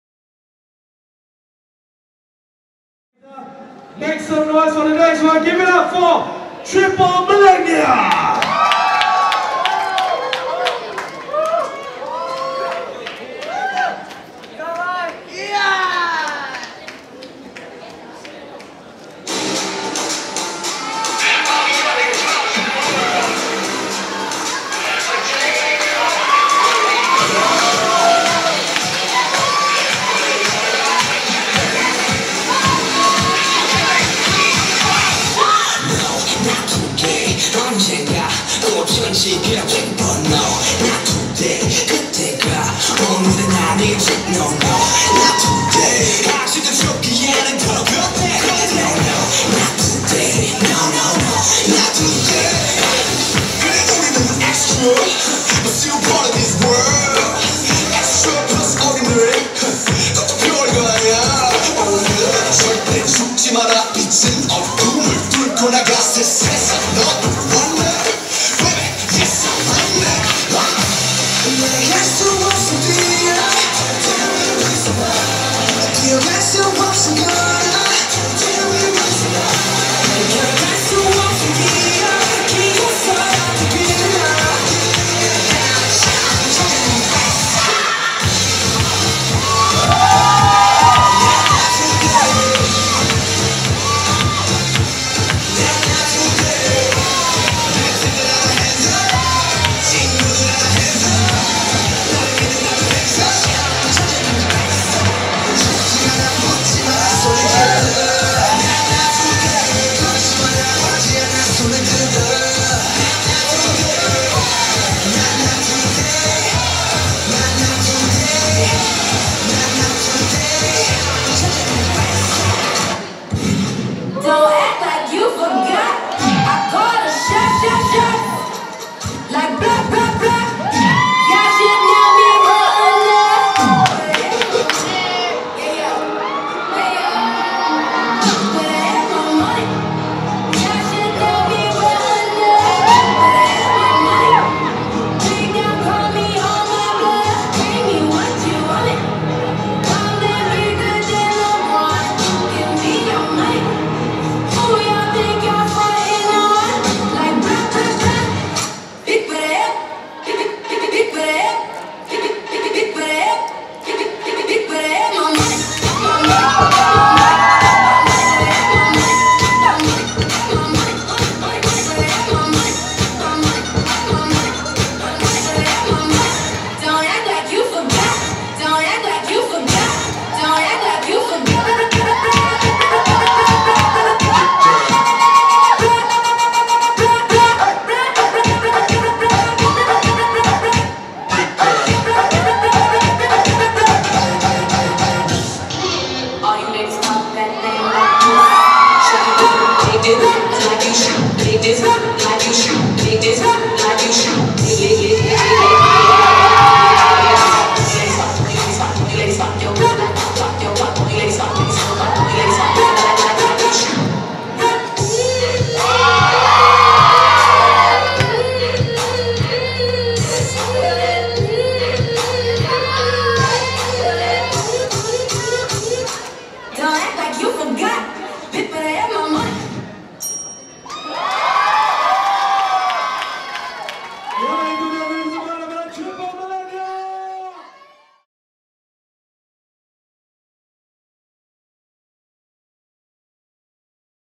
TRIPLE MILLENIA : REMAJA DANCE OFF 2018
Lit tak performance dari Triple Millenia?

REMAJA DANCE OFF 2018
Dibawakan oleh Nu Ideaktiv & Remaja.
Lokasi: Misfit 2018, PWTC Kuala Lumpur.
Tarikh: 5 Ogos 2018